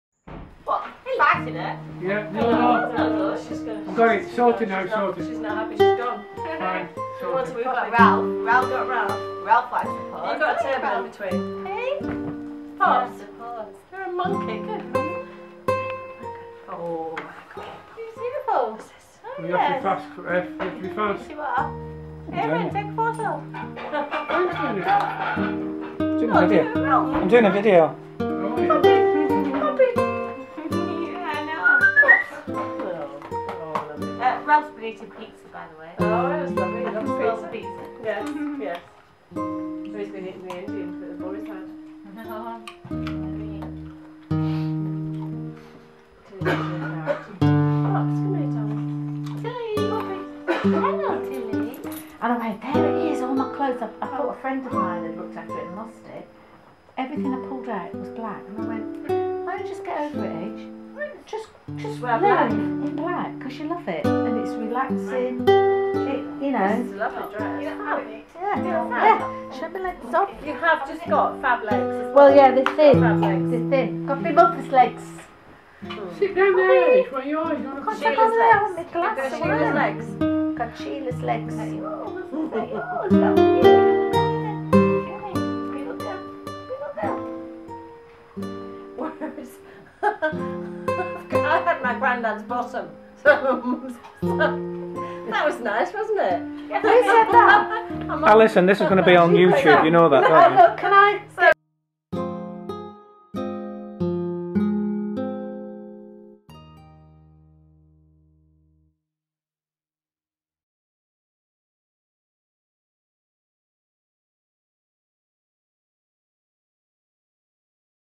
At Home with the Sealyhams at Seasmolt
Sealyhams Ralph, Poppy and Tilly, Sealyhams enjoying life playing in front of an open fire and eating CARROTS! Seasmolt